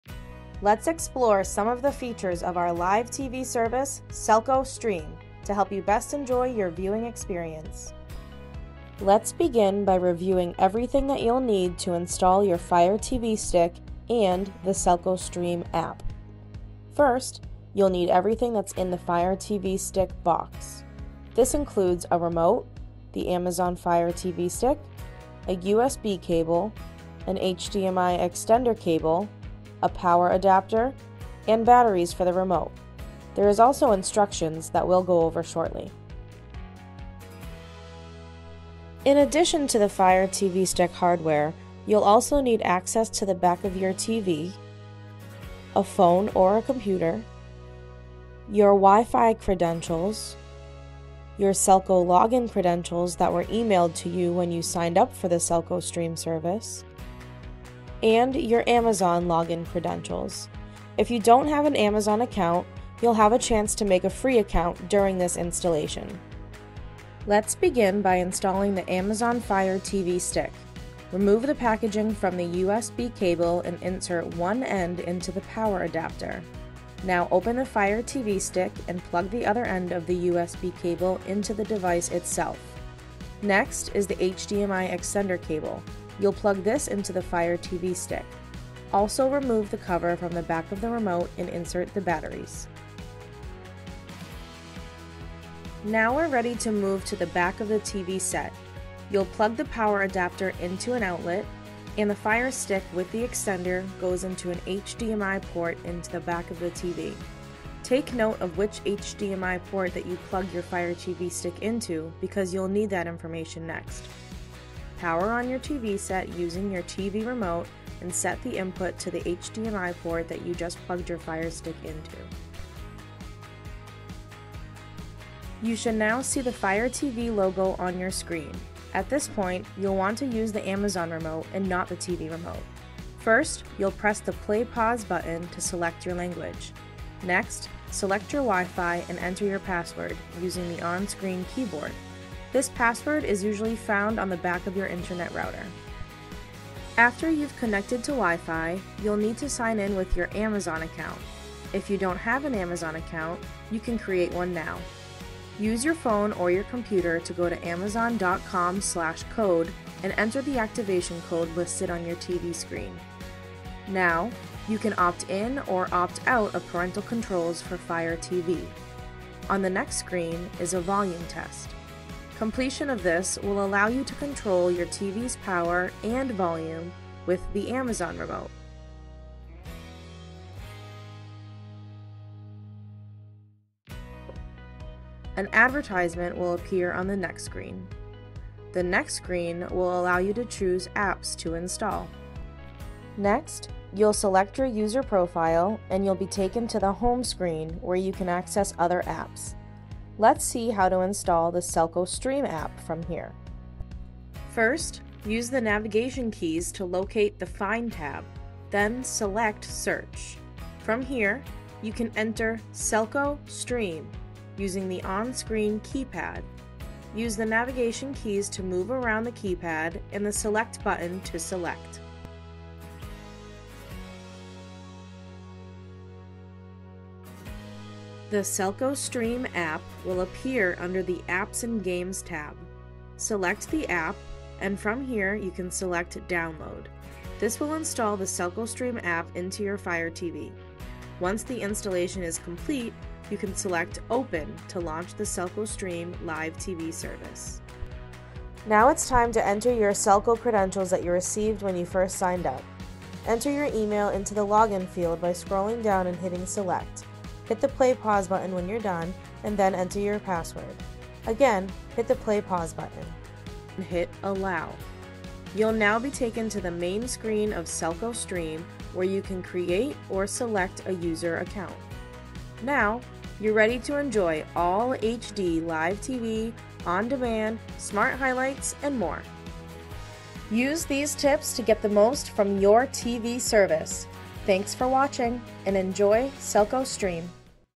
Fire Stick & SELCO Stream Installation
Explore the features of SELCO Stream, SELCO's Live TV Streaming Platform. Here, we'll review how to install your Amazon Fire TV Stick and the SELCO Stream App.